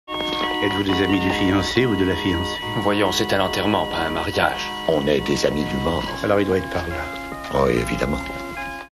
On est des amis du mort ...
Etes-vous des amis du fiancé ou de la fiancée ? Voyons c'est un enterrement pas un mariage. On est des amis du mort. Alors il doit être par là.